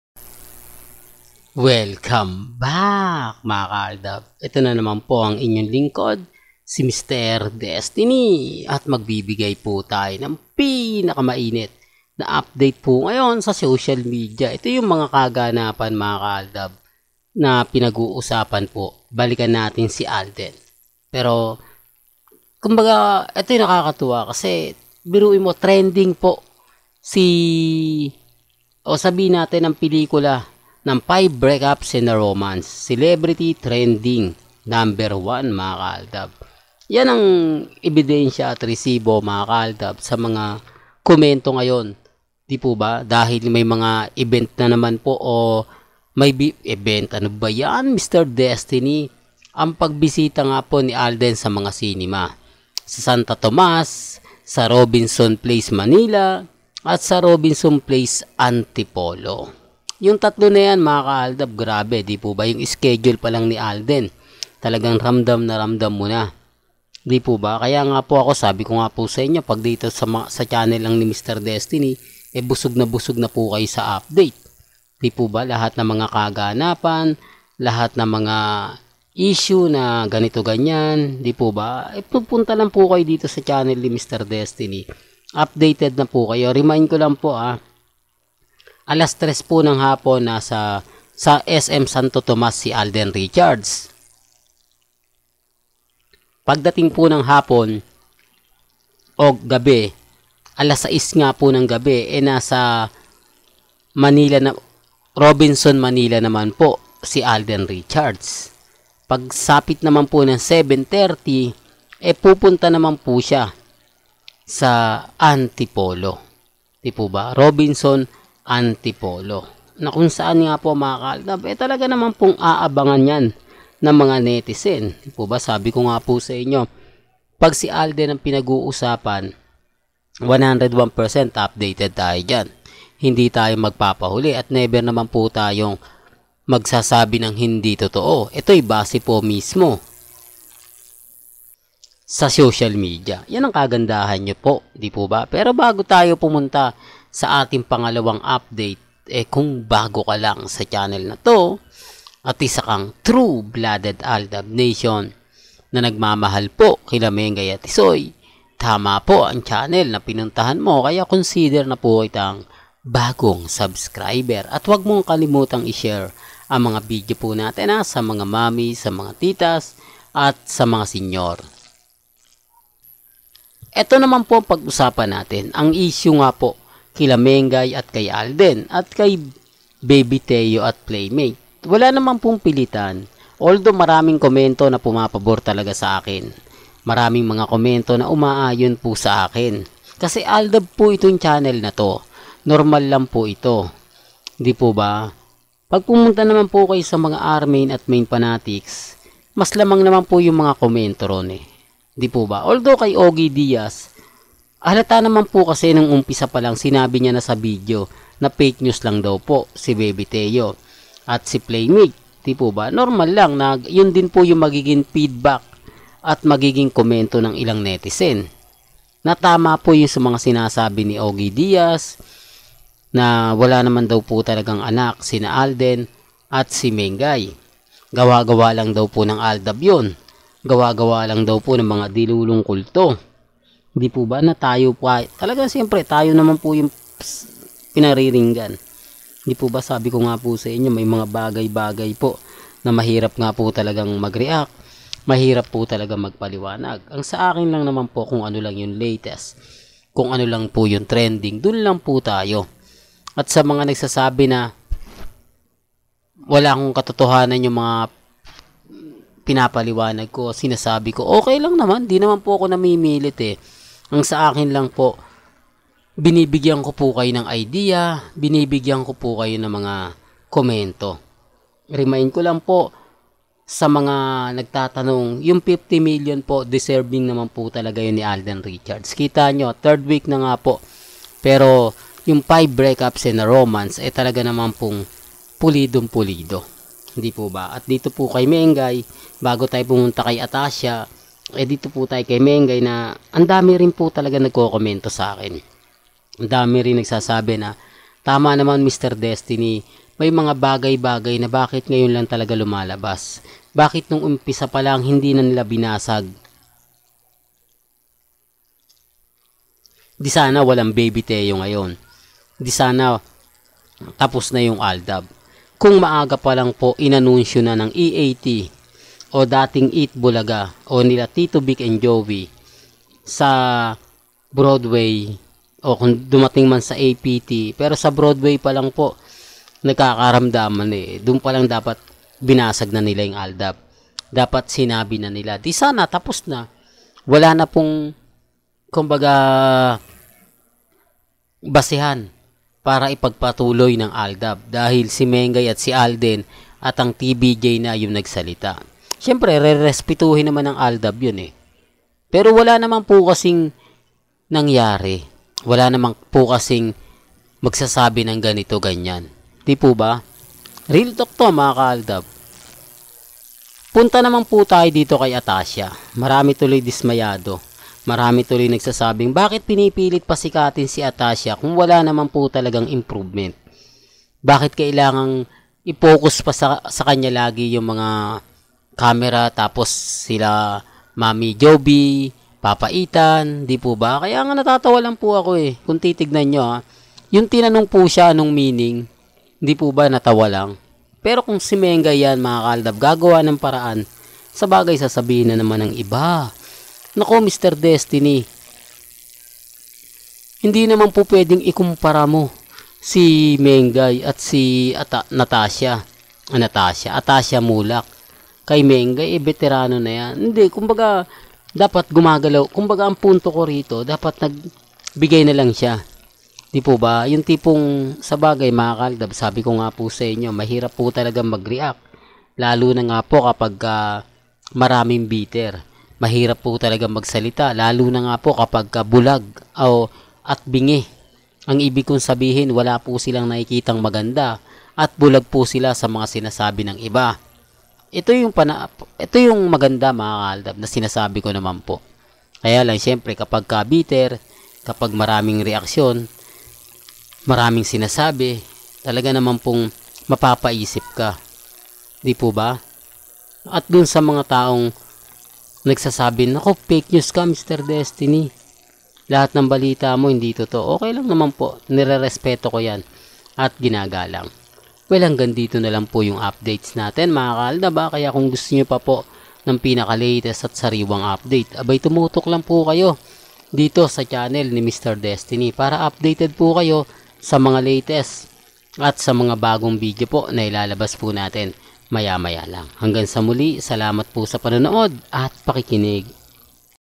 LATEST UPDATES Ngaun kina Maine Mendoza & Alden Richards at Atasha Muhlach binabatikos dahil sa hind
LATEST UPDATES Ngaun kina Maine Mendoza & Alden Richards at Atasha Muhlach binabatikos dahil sa hindi pagsagot